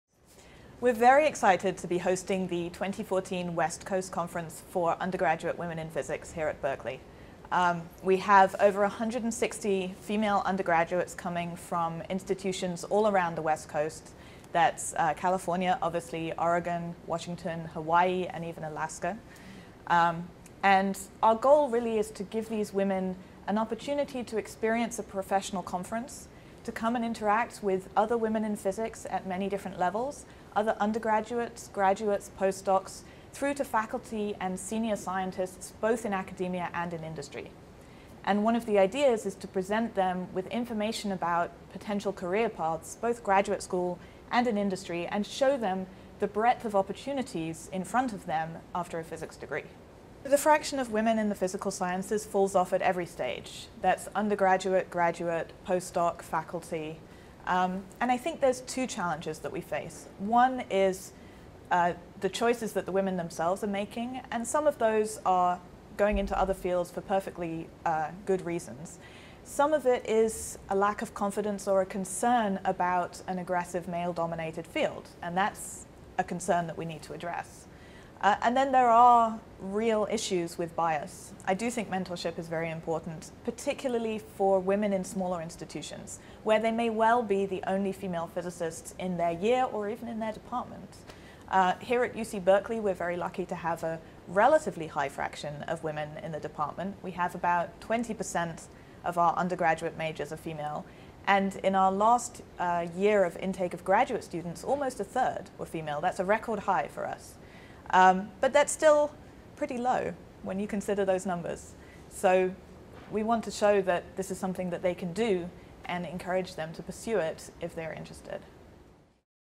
Berkeley wants More Women in Physics | Prof. Gabriel Orebi Gann
Berkeley hosts the West Coast Conference for Undergraduate Women in Physics, a three-day event (January 17-19, 2014), designed to encourage attendees to pursue careers in a field where women are still a minority. Talks, panel discussions, lab tours and a career fair will be held the University of California, Berkeley, Lawrence Berkeley National Laboratory (LBNL) and Lawrence Livermore National Laboratory (LLNL).

The conference, which will draw nearly 170 women from the western U.S. who major in physics and related fields, is one of eight concurrent regional conferences sponsored by the American Physical Society that attract about 1,000 women nationwide.